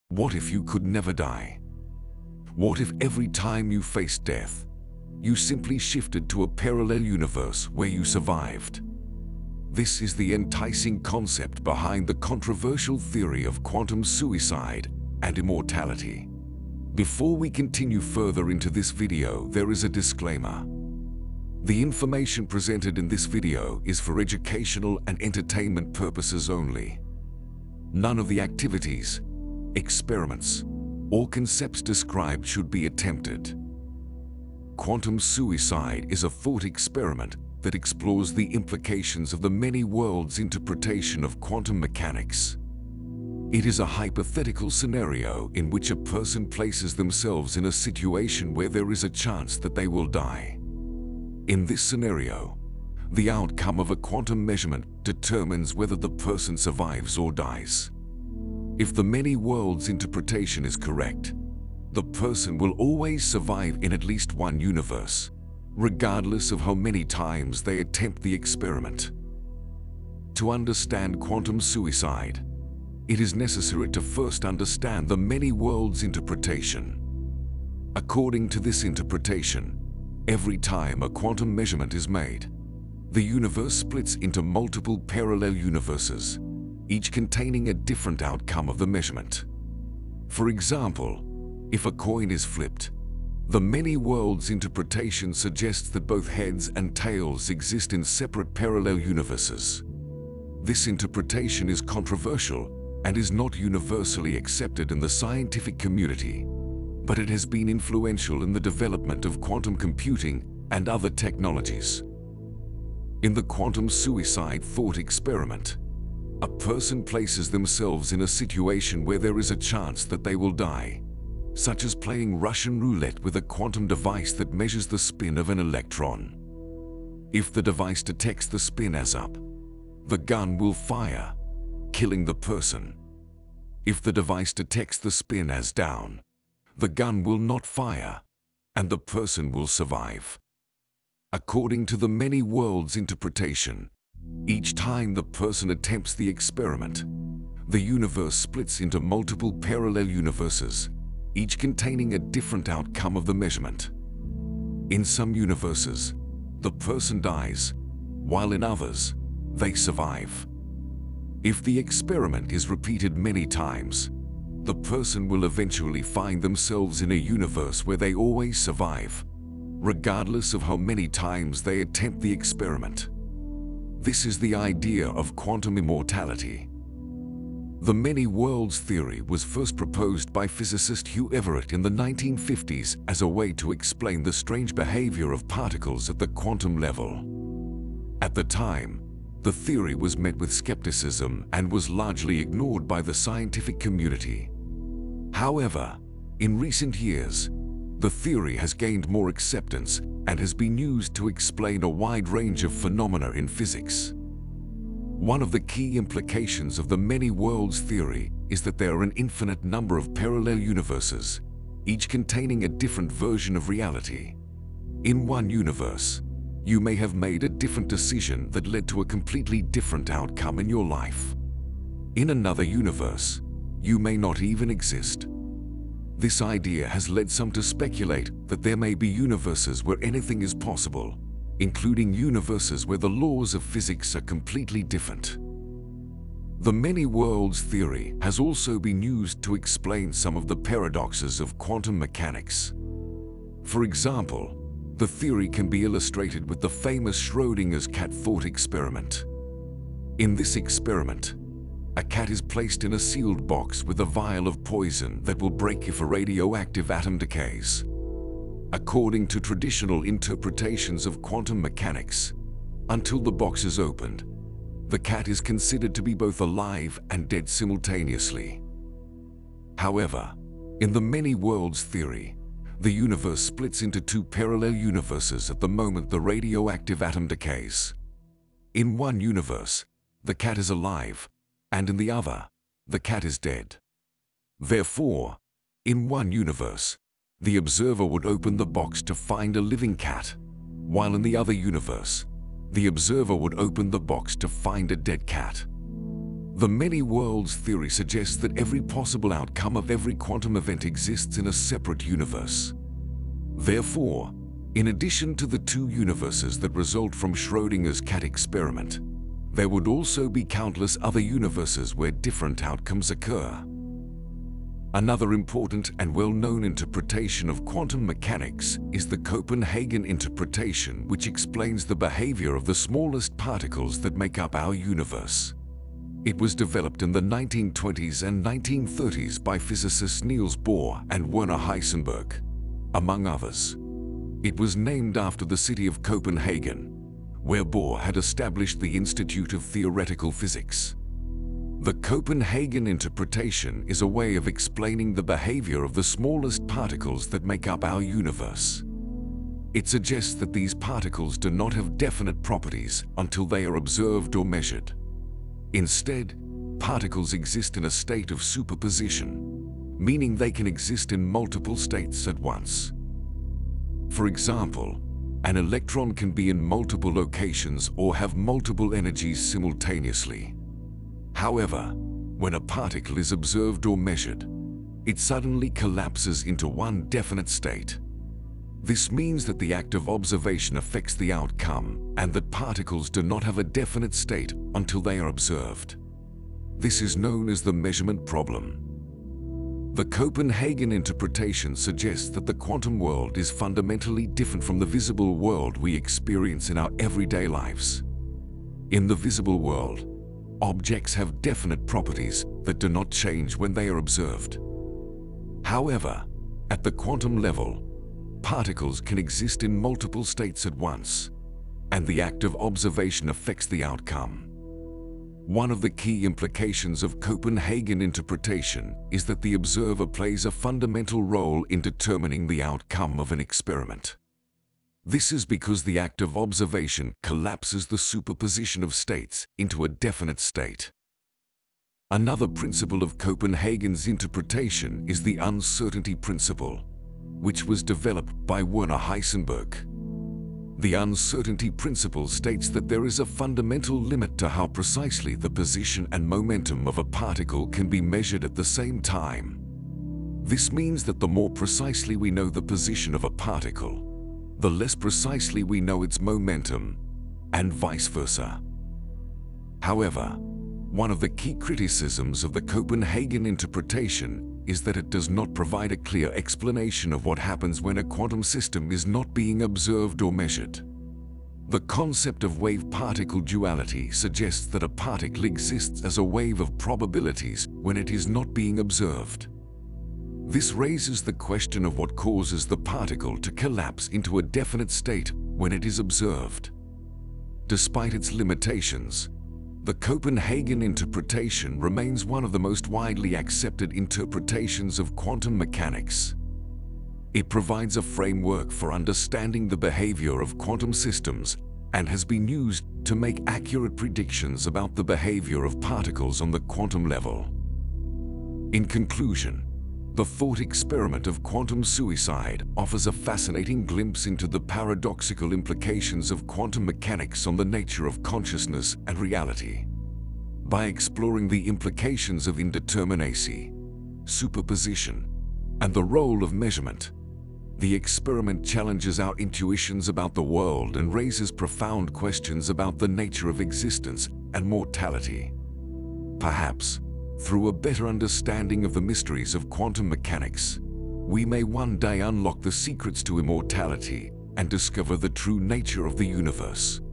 Proof Of Immortality And Parallel Universes According To Quantum Physics
Quantum Suicide - 0:00
Many-Worlds Theory - 1:15
Schrödinger's Cat - 4:07
Behaviour of the Smallest Particles - 5:15
Heisenberg’s Uncertainty Principle - 7:12